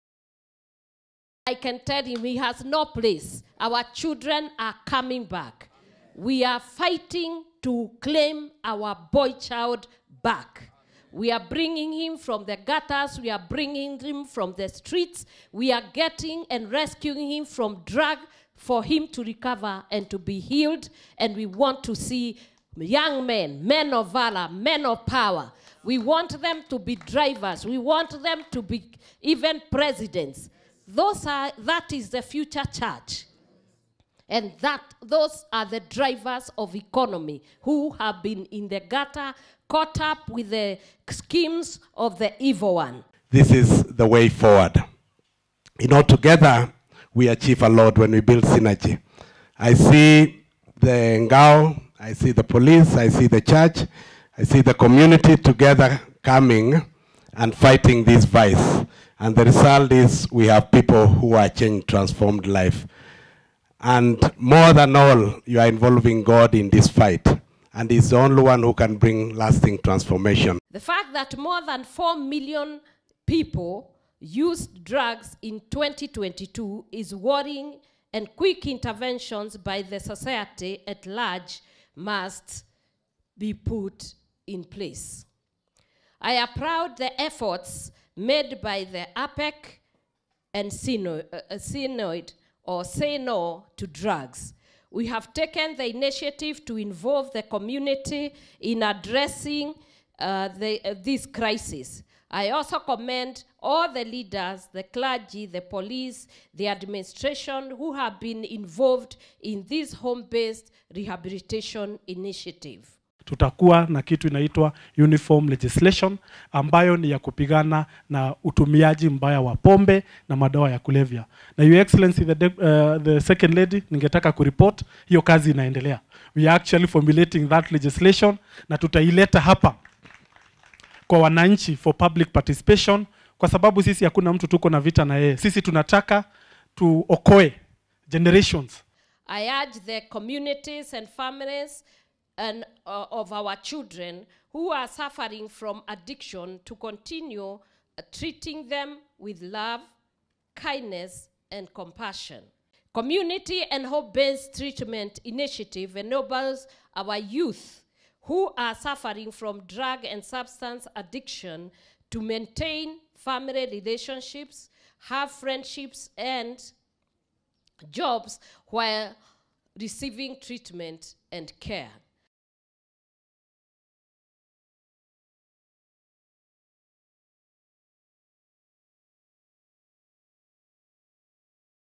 Pastor Dorcas Rigathi: Every Addict Deserves To Rebuild Their Lives
The Spouse of the Deputy President Pastor Dorcas Rigathi has called on the families of those in addiction to embrace and treat them with love, kindness, and compassion to sail through the healing process.
Speaking in Githunguri Sub-County Kiambu County when she toured the Say No to Drugs home-based care center (Saynod), Pastor Dorcas emphasized that the involvement of the community and home-based rehabilitation will go a long way in eliminating drugs and substance abuse among the youth.

“Community and home-based treatment initiatives enable our youth who are suffering from drugs and substance addiction to maintain family relationships, have friendships and jobs while receiving treatments and care. Addiction is an illness whose healing is fast-tracked by the support one receives from those who are close to them. “She said.